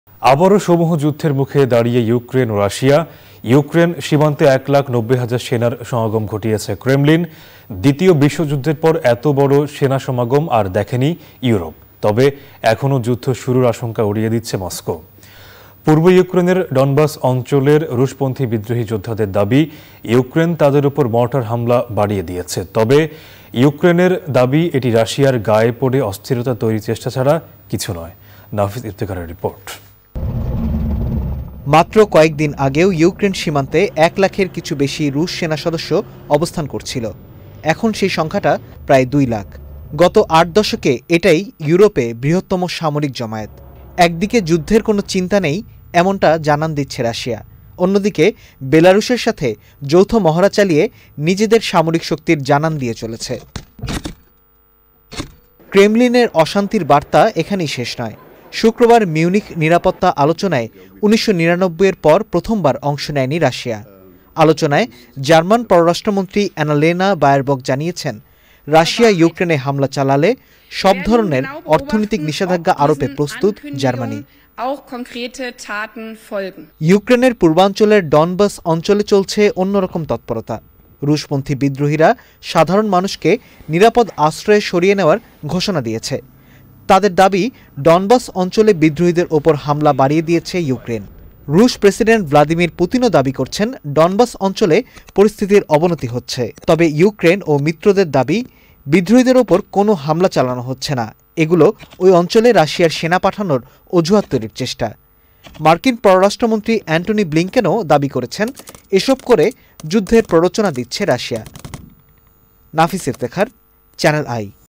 ইউক্রেন সীমান্তে রাশিয়ার রেকর্ড সেনাসমাগম
আবারও সমূহ যুদ্ধের মুখে দাঁড়িয়ে ইউক্রেন ও রাশিয়া। ইউক্রেন সীমান্তে ১ লাখ ৯০ হাজার সেনার সমাগম ঘটিয়েছে ক্রেমলিন। দ্বিতীয় বিশ্বযুদ্ধের পর এত বড় সেনা-সমাগম আর দেখেনি ইউরোপ। তবে এখনও যুদ্ধ শুরুর আশঙ্কা উড়িয়ে দিচ্ছে মস্কো। পূর্ব ইউক্রেনের ডনবাস অঞ্চলের রুশপন্থী বিদ্রোহী যোদ্ধাদের দাবি ইউক্রেন তাদের ওপর মর্টার হামলা বাড়িয়ে দিয়েছে। তবে ইউক্রেনের দাবি এটি রাশিয়ার গায়ে পড়ে অস্থিরতা তৈরির চেষ্টা ছাড়া কিছু নয়। নাফিস ইফতেখারের রিপোর্ট।

worldwide.